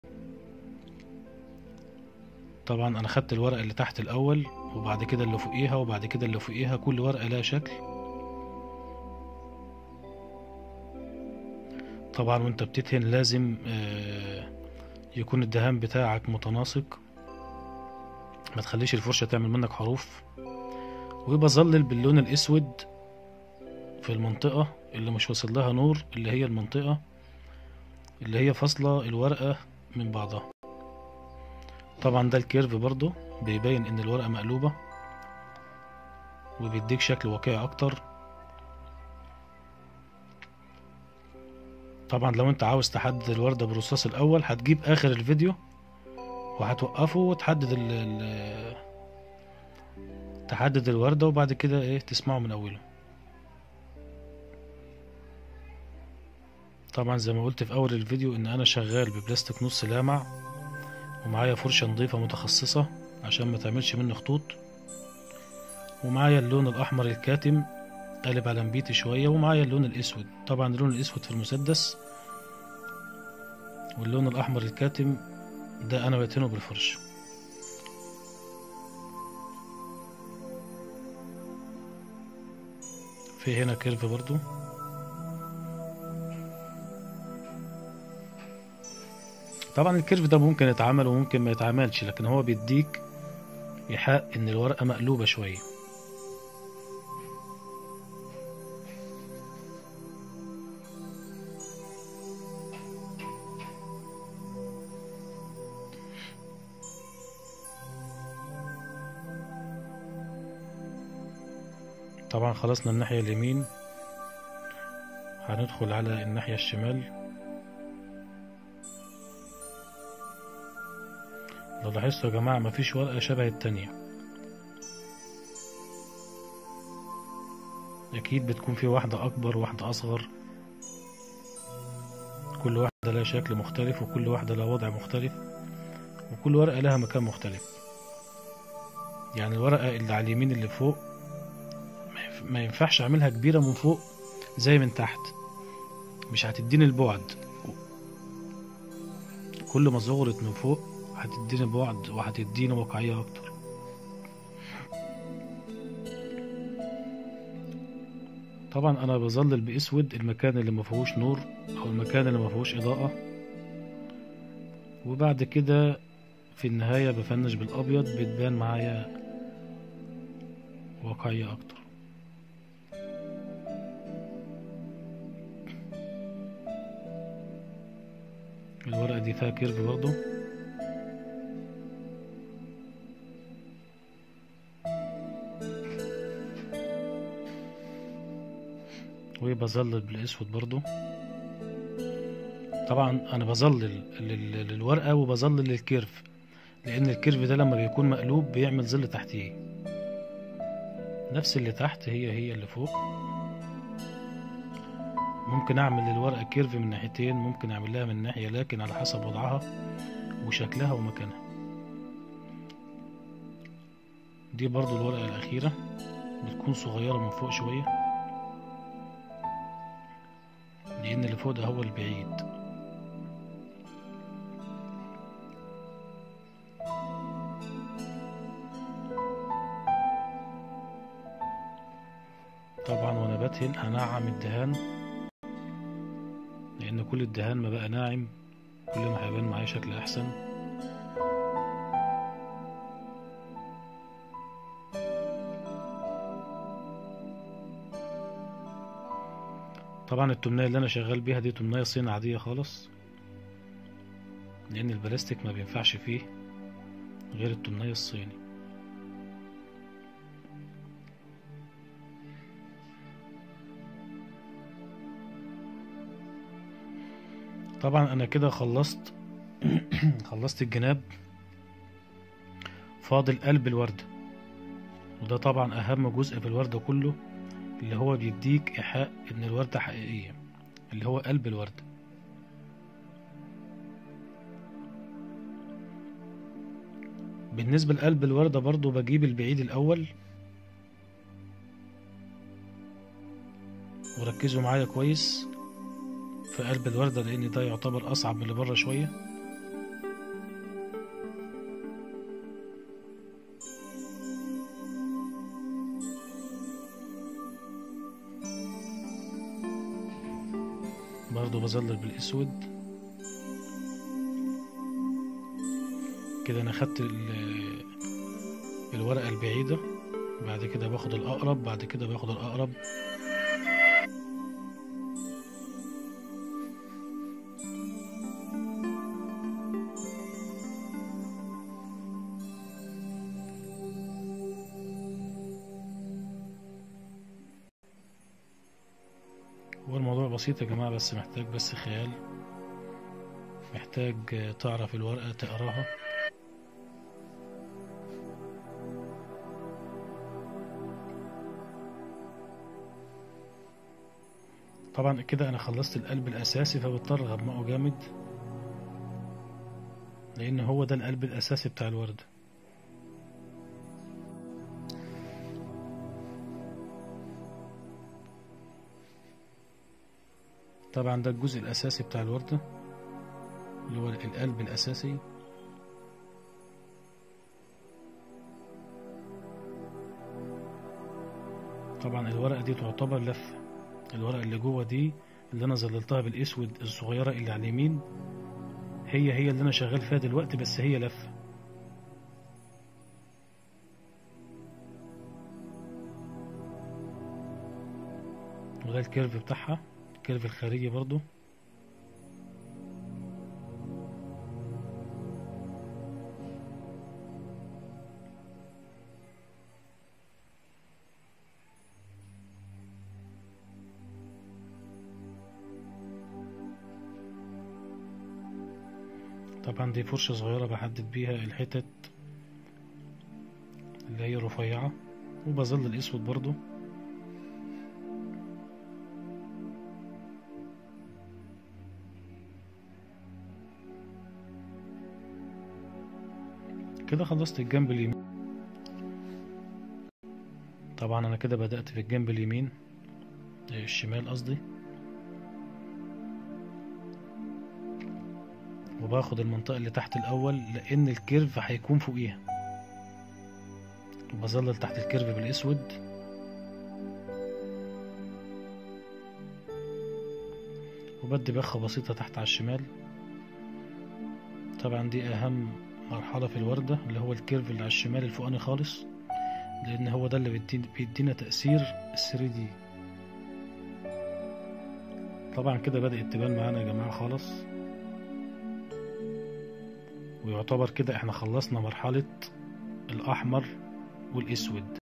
Rose painting on wall by hand
I teach painting for beginners
making flower on wall
required tool 2inch brush water matt red colour and air tools